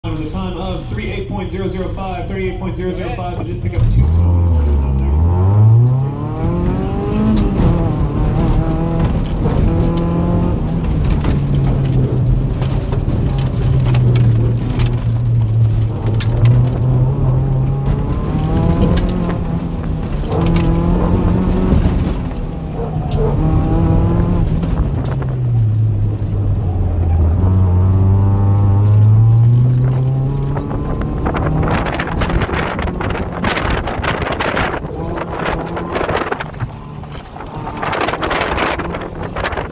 HSCC - Night Series 3 - 2019
Quickie from the 3rd event of 2019. That mirror is holding on for dear life.